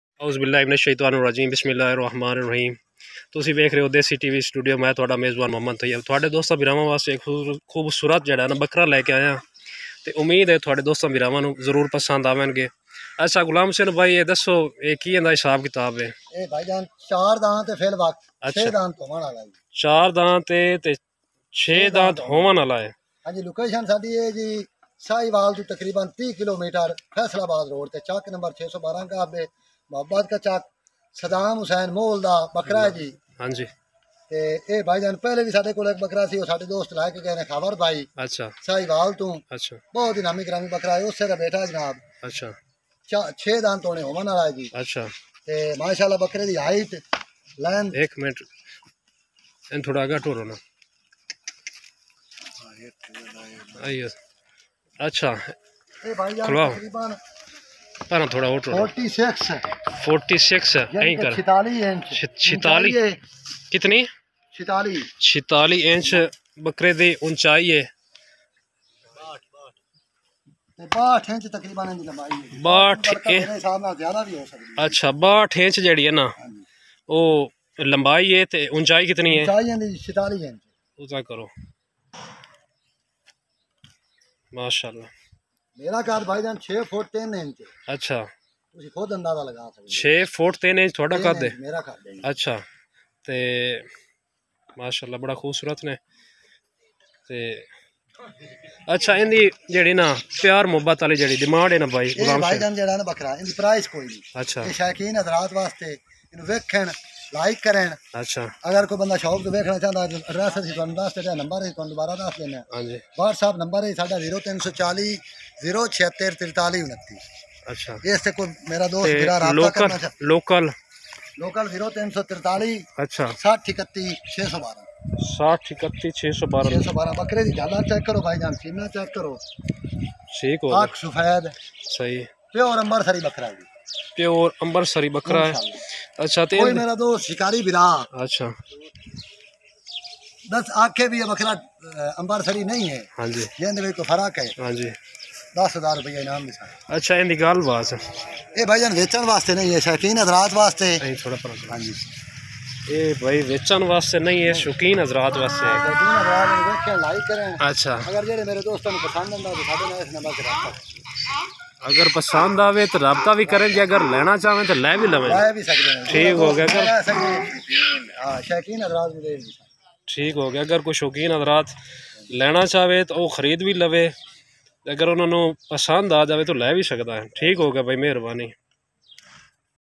Worlds most expensive goat ever_world most most breed guabi......10 million goat ever....Arabi bakra
Yeh bakara kurbani k liyeee hai bahot nayab kism ka ha
# desi tv studio
Muhammad tayyab
most expensive goat
location 614 GB